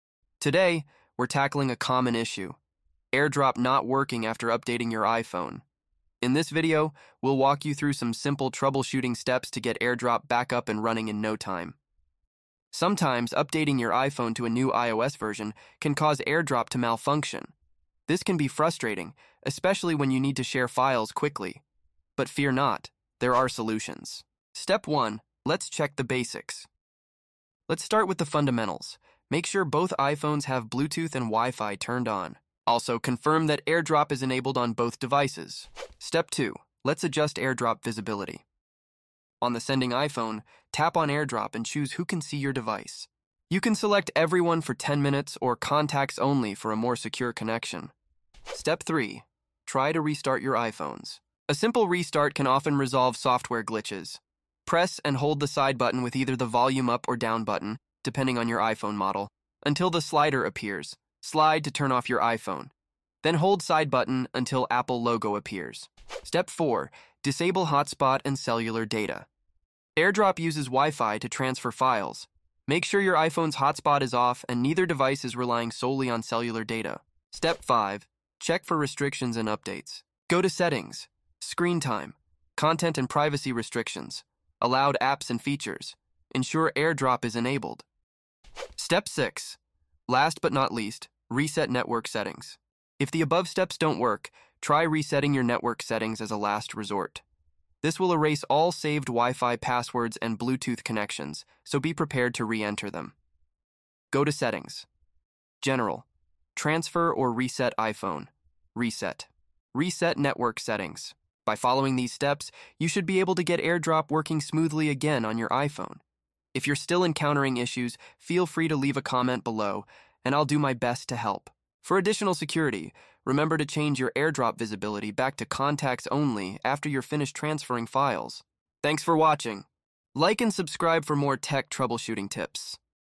iOS 17.5 - How To Fix Airdrop On iPhone After Update!
Learn How To Fix Airdrop On iPhone After Update!

Is AirDrop not working after an update on your iPhone? 📲 In this video, we'll show you how to troubleshoot and fix AirDrop issues step-by-step. From checking your settings to resetting connections, we'll help you get AirDrop back up and running smoothly. Share files effortlessly again!